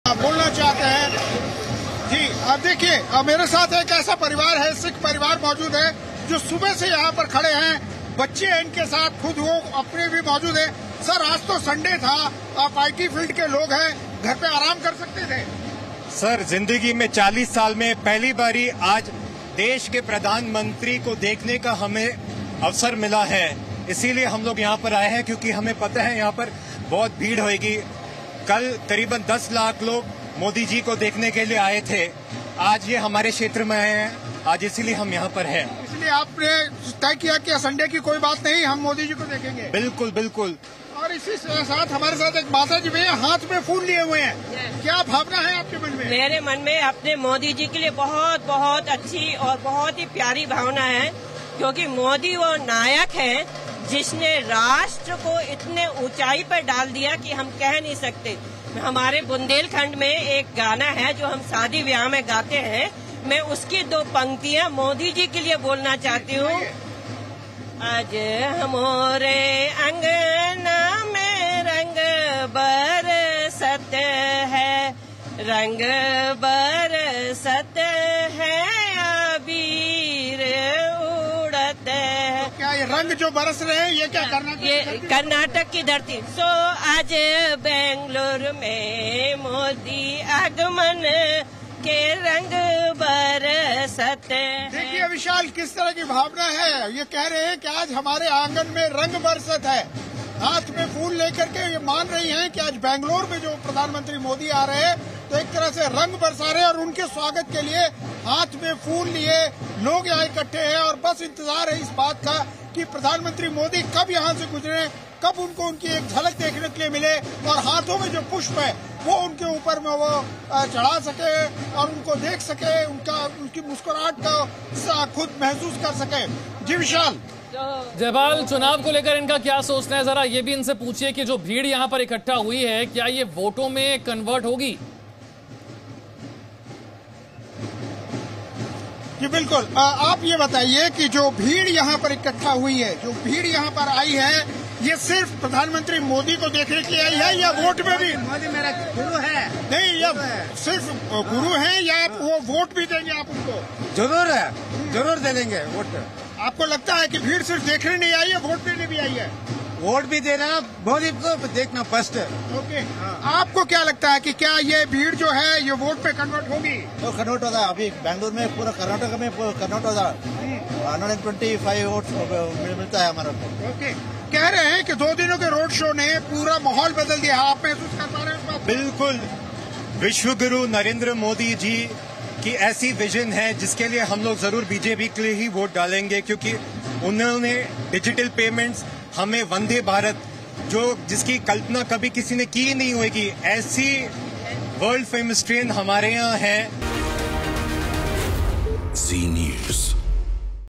PM Modi road show: बेंगलुरु की सड़कों पर मोदी-मोदी के नारे, रोड शो में उमड़ा जनसैलाब
कर्नाटक चुनाव से पहले बीजेपी ने आखिरी दांव चला है. बेंगलुरु में पीएम मोदी का 10 किलोमीटर लंबा रोड शो शुरू हो गया है.

The BJP has played its last bet before the Karnataka elections. PM Modi's 10 kilometer long road show has started in Bengaluru.